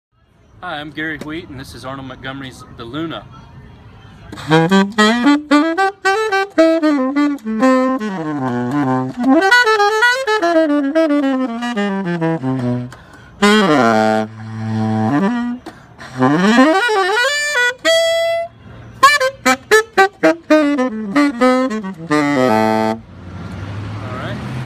Gary Wheat
Gary Wheat is a Birmingham, AL based saxophonist and recording. Gary is a phenomenal player. Gary was kind enough to try out one of my Luna Tenor saxophone mouthpiece prototypes.